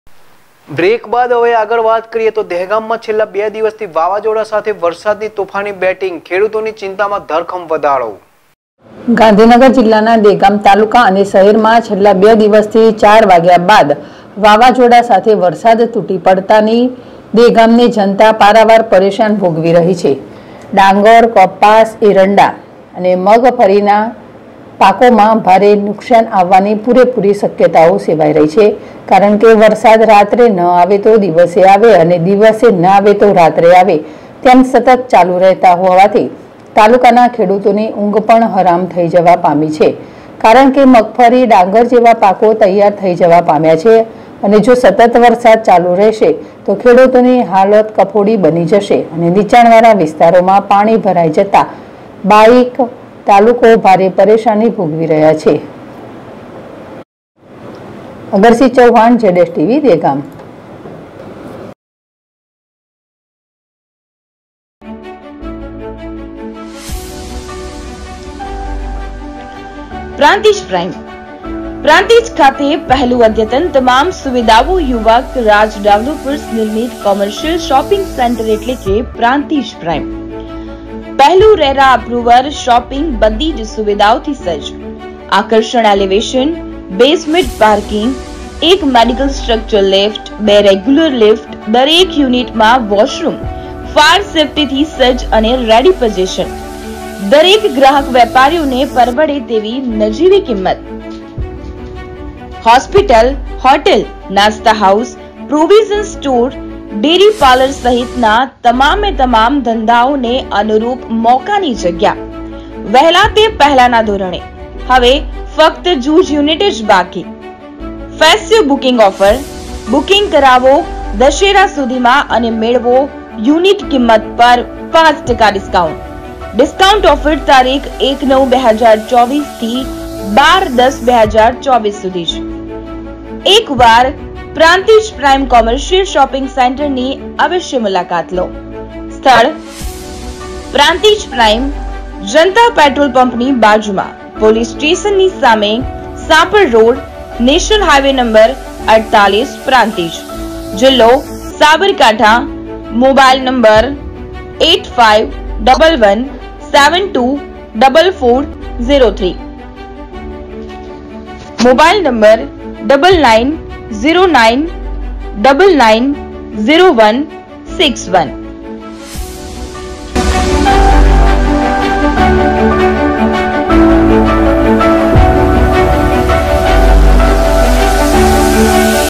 દહેગામ તાલુકાની શહેરમાં છેલ્લા બે દિવસથી વાવાઝોડા સાથે વરસાદની તોફાની બેટિંગ ખેડૂતોની ચિંતામાં
દહેગામ તાલુકાની શહેરમાં છેલ્લા બે દિવસથી વાવાઝોડા સાથે વરસાદની તોફાની બેટિંગ ખેડૂતોની ચિંતામાં ધરખમ વધારો